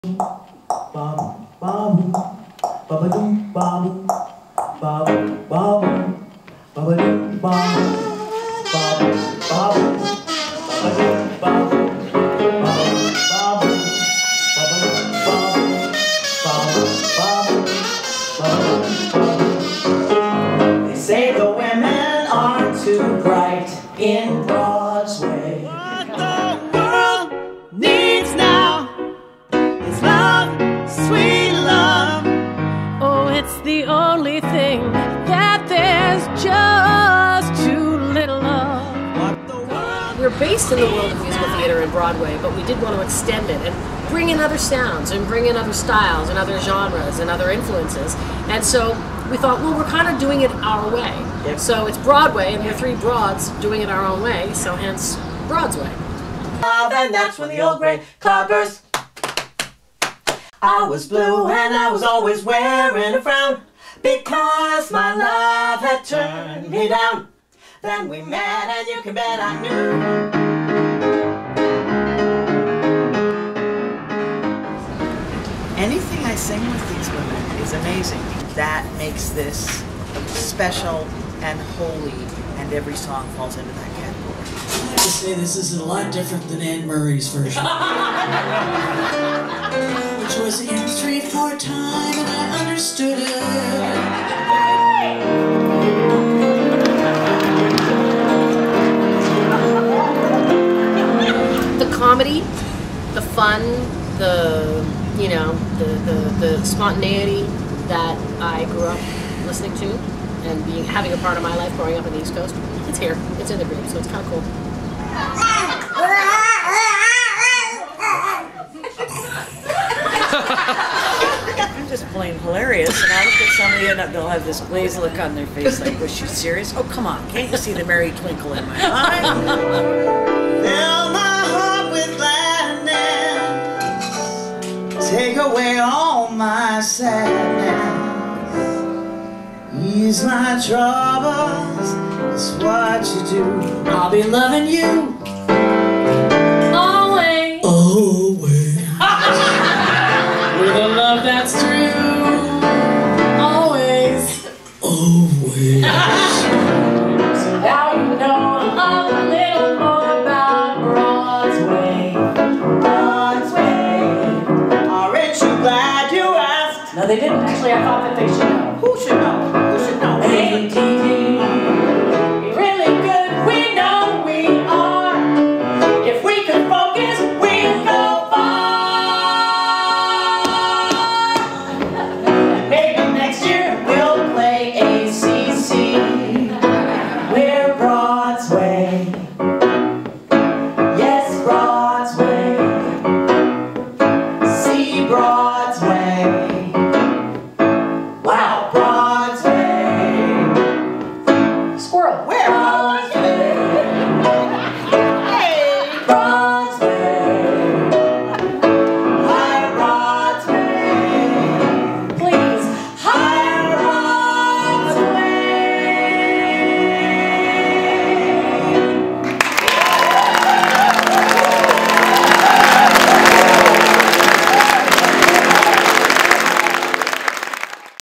BROADSWAY EPK VIDEO
From Gershwin to Gaga, three of Canada's finest musical theatre, cabaret & jazz artists doing Broadway their way!